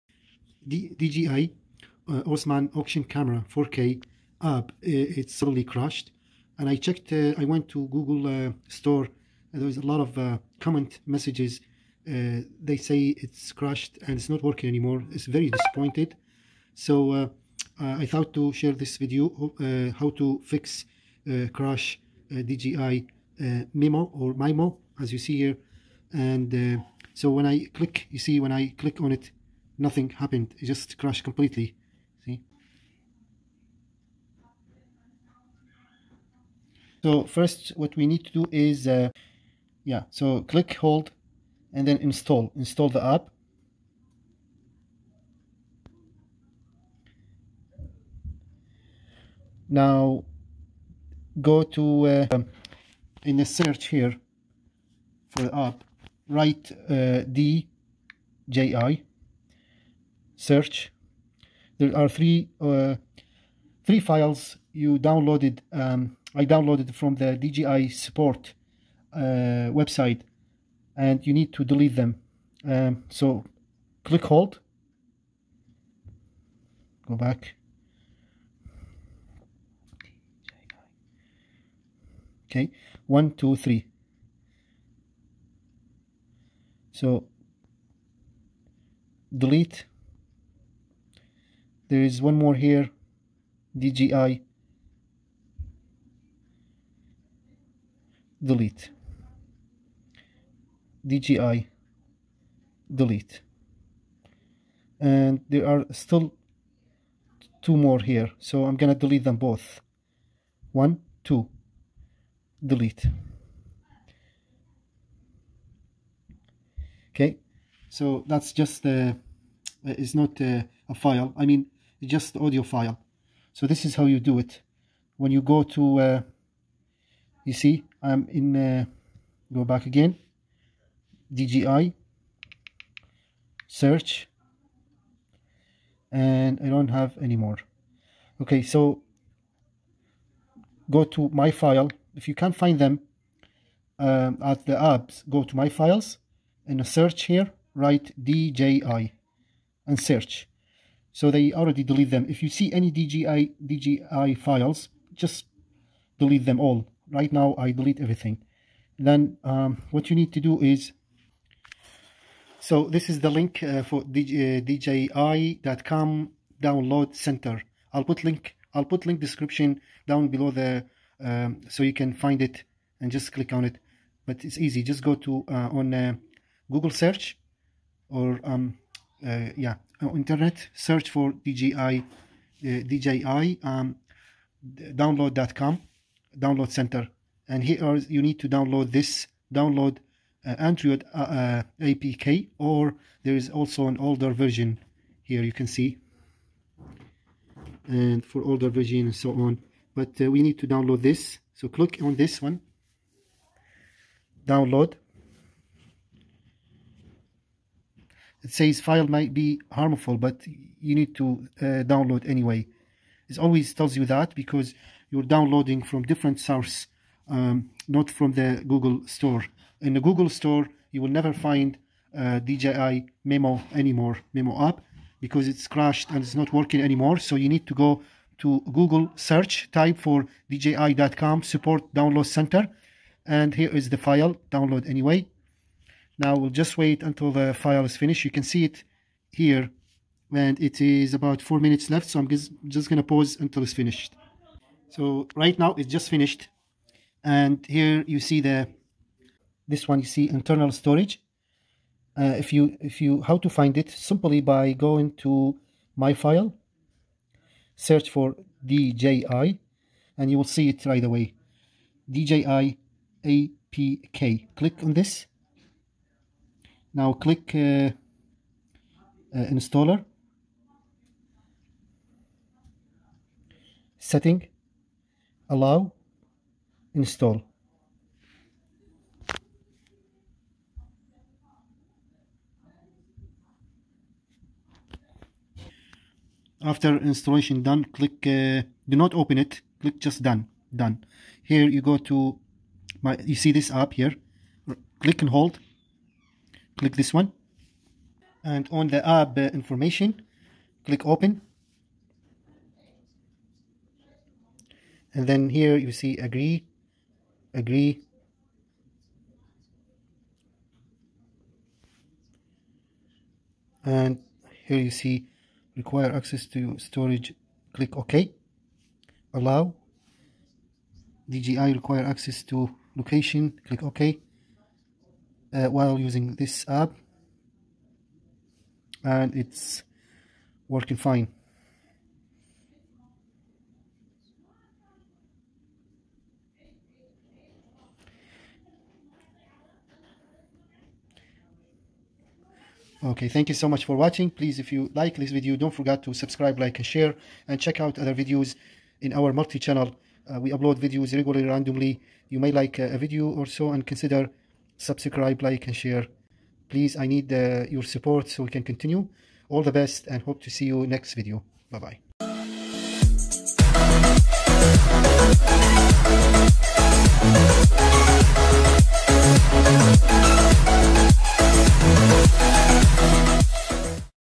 DJI Mimo App Won't Open Anymore (Crashed) Solved, How To Fix DJI App Mimo Stopped working, Easy Fix
everytime anderiod updates to newer versions dji app stopped working anymore because dji can't fix thier apps so they work with Android systems new updates, dji don't have the best technology with Android systems yet,

this video shows you how to fix this problem and make it work like a magic again,

1- uninstall dji App
2- delete all apk versions you downloaded from dji.com download center
3- install new version apk from dji website download center,
4- watch this video and follow the instructions
done and enjoy 😉

Hello my friends, if anyone has problems
Try those steps
1- go to my files and check option it says (show hidden files)
Search for dji app, dji mimo, dji apk and Delete any related dji files and folders completely
2- restart your phone
3- check for any update your phone software software if any  new version available anderoid version 12 should work
4- go to dji download center website and download mimo app apk new version when you say pop up a message says (this app may harmful your device) ignore this message and allow to continue download from phone setting security and allow permission process,
5- don't open app when it's finished close it and go to app mimo icon and press hold and on pop up message box press on (¡) and on the bottom bar  of the screen you see 3 options press on (open) done
Enjoy and all the best 👌 👍